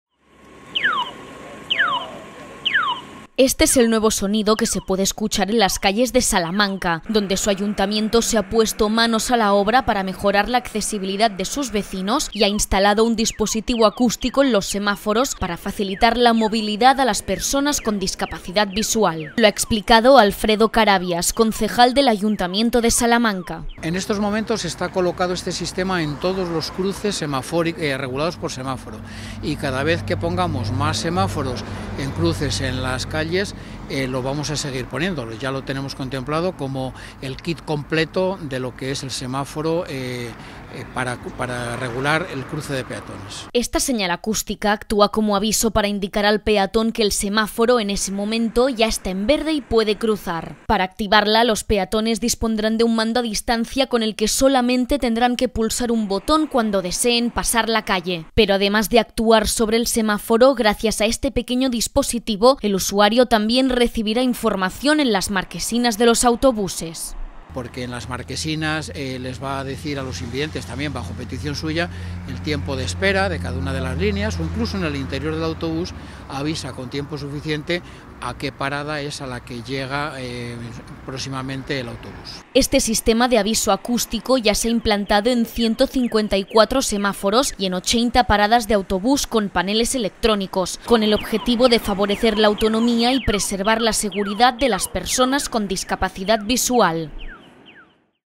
V Salamance mají semafory s akustickým signálem pro slepce
Městská rada v Salamance nainstalovala na všechny semafory ve městě systém dálkového ovládání, který po aktivaci upozorní lidi se zrakovým postižením, když se rozsvítí zelená a mohou přejít.
EVANews